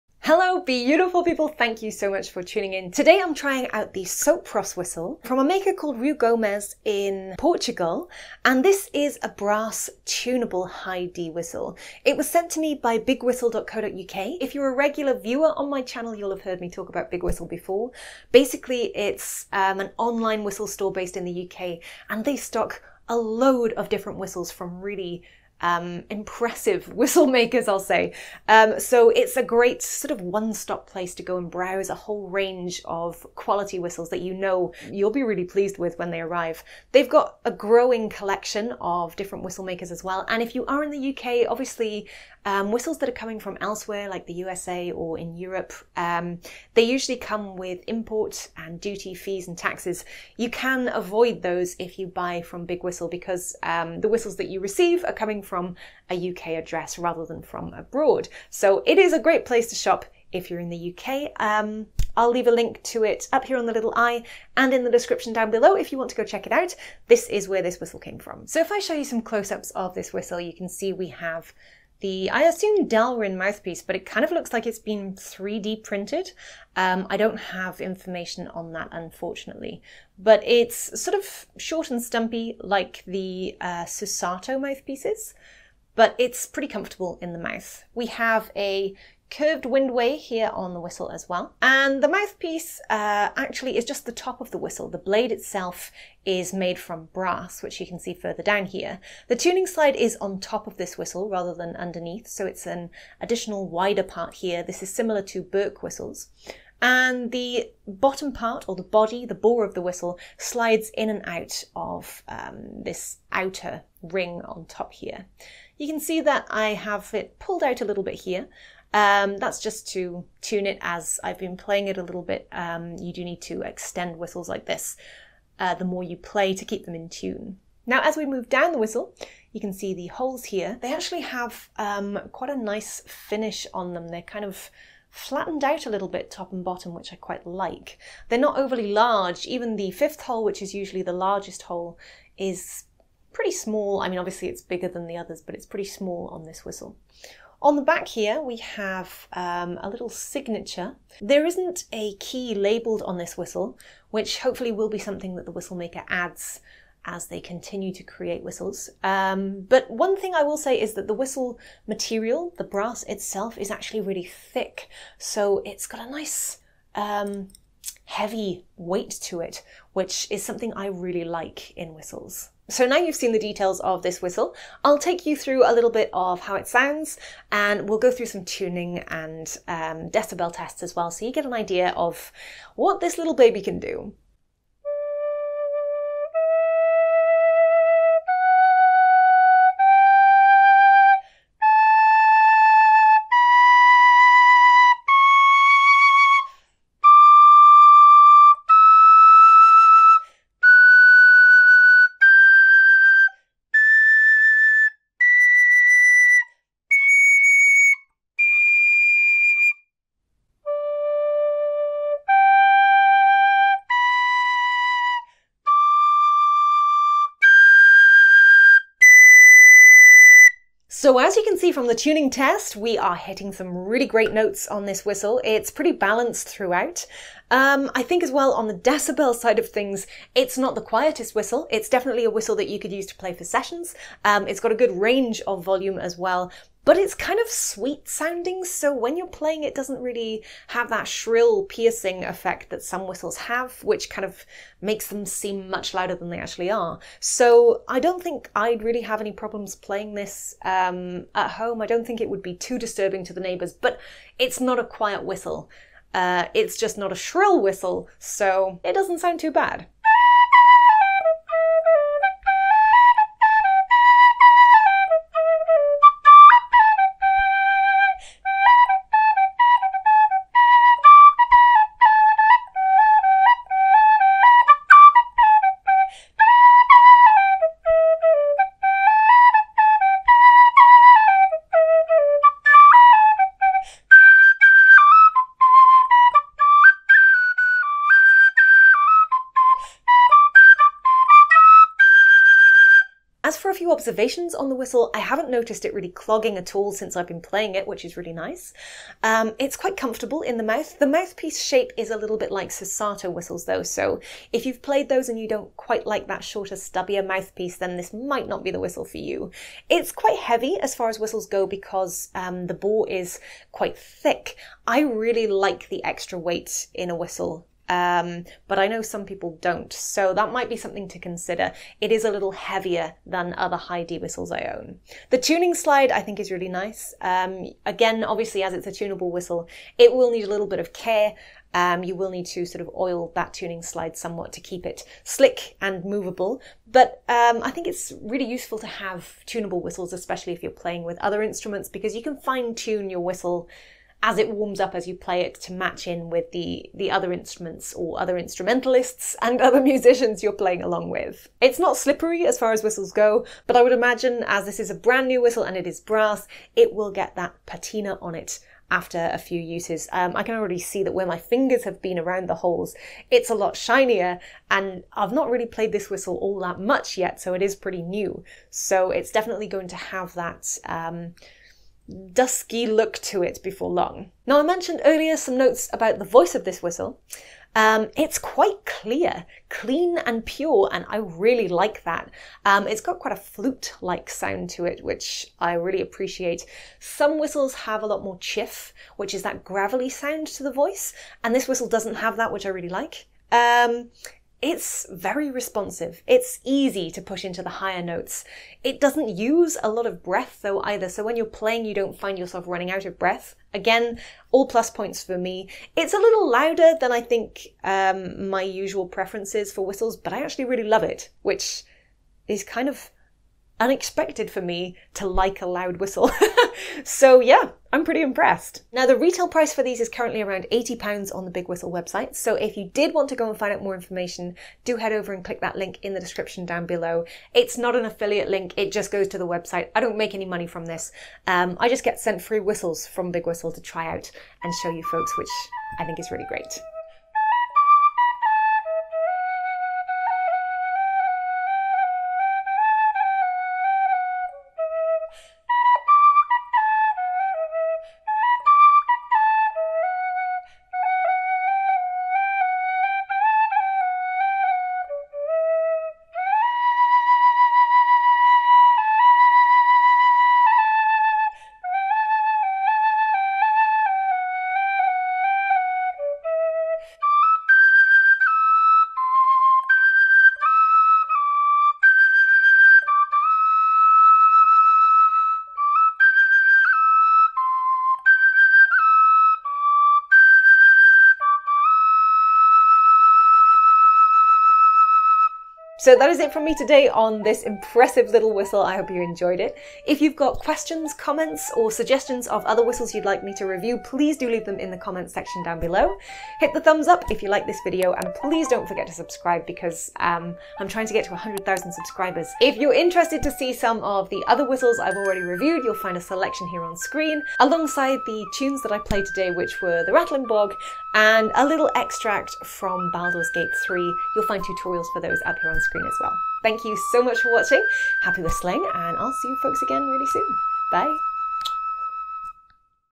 SOPROS BRASS HIGH D WHISTLE - By Rui Gomez | Tin Whistle Review
Today I'm trying out the brand new Sopros Whistle by Rui Gomez, a Portuguese whistle maker. It's a brass high D whistle, and it's tuneable too. This one came from BigWhistle.co.uk - find out more here:

00:00 - Intro
01:08 - Whistle Details
02:52 - Tuning & Volume
04:20 - Raw Sound Sample
04:53 - Pros/Cons
06:58 - Price & Purchase
07:17 - Raw Sound With Reverb
08:05 - Links